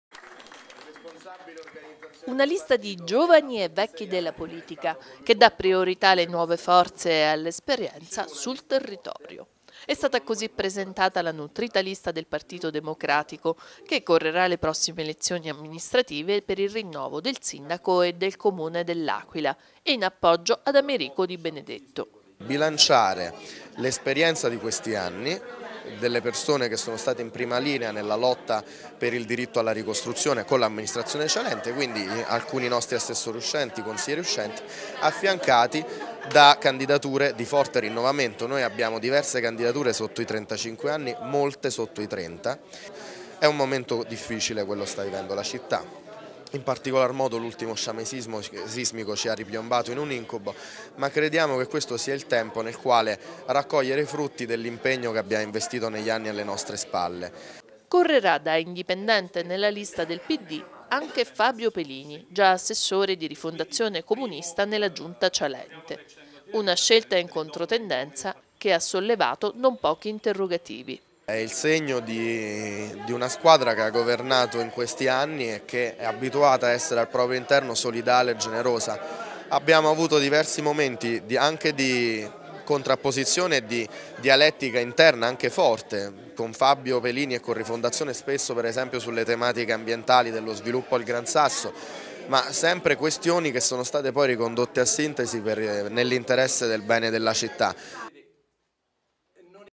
L'Aquila. Il Pd presenta le sue liste
Con l'assenza di Cialente, sebbene giustificato da impegni istituzionali, il partito democratico ha presentato la sua lista per le prossime elezioni